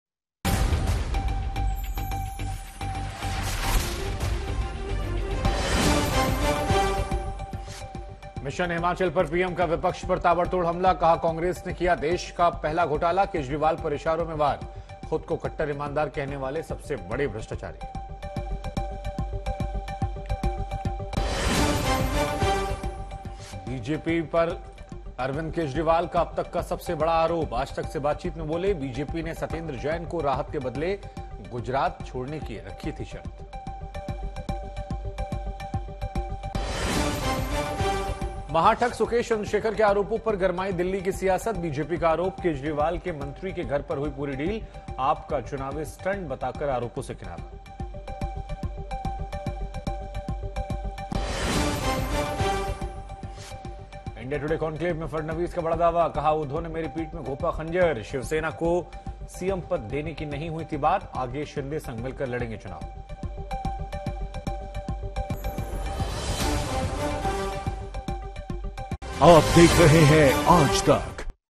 Top Headlines Of The Day: Devendra Fadnavis | Gujarat Elections 2022 | Latest News
Devendra Fadnavis ने बताया- पुरानी सरकार सुबह-शाम झूठ ही बोलती थी | India Today Conclave

देवेंद्र फडणवीस से पूछा गया कि 2024 में दोबारा चुनाव होगा तो सीएम फेस कौन होगा? इस पर उन्होंने कहा कि एकनाथ शिंदे और हम मिलकर चुनाव लडेंगे. फेस कौन होगा, इसका महत्व नहीं है क्योंकि हमारे पास हुकुम का इक्का है, वह हैं- पीएम मोदी. उनके पीछे कोई भी फेस लगा दीजिए, सब आगे बढ़ जाते हैं.

Devendra Fadnavis was asked who will be the CM face if there is a re-election in 2024? On this he said that Eknath Shinde and we will fight the elections together. Who will be the face, it does not matter because we have the ace of spades, that is - PM Modi. Put any face behind them, everyone moves forward.